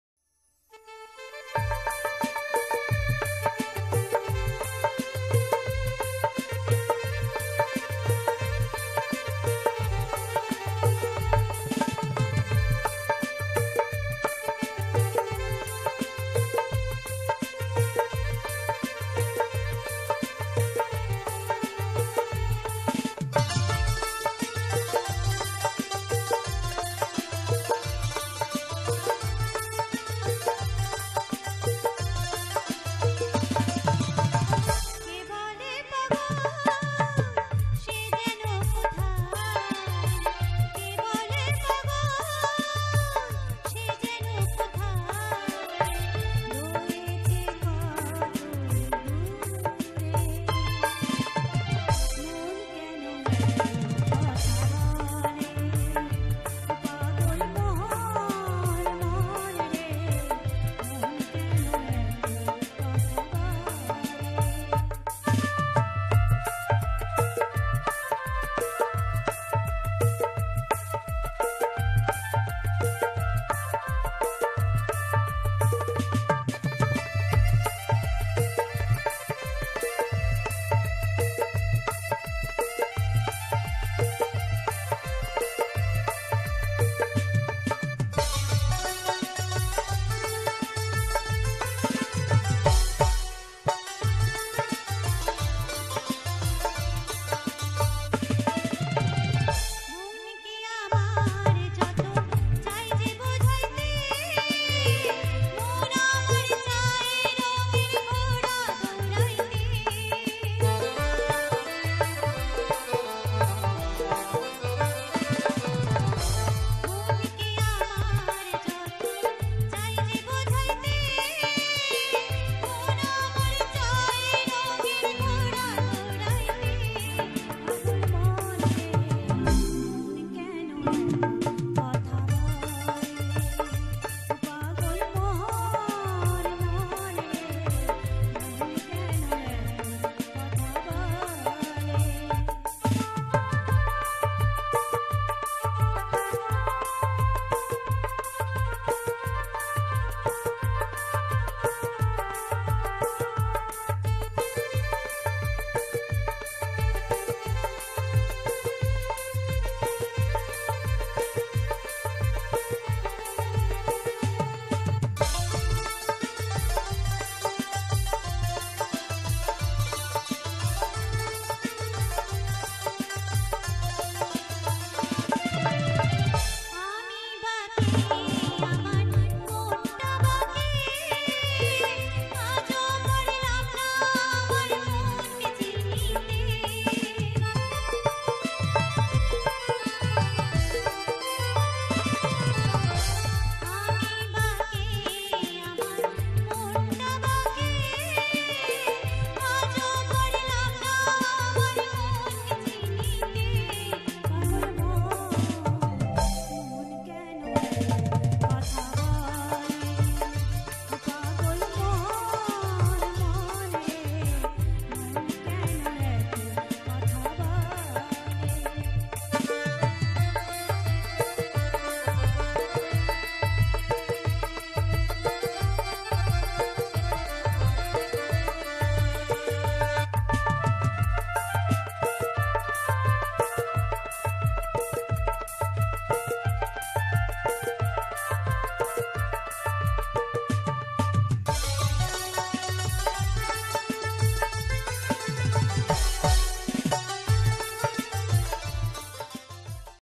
Here's my only wife singing for you all a Bengali folk on the majestic platform Govt of West Bengal.
Video from Àrindam Kumar::

Let's enjoy.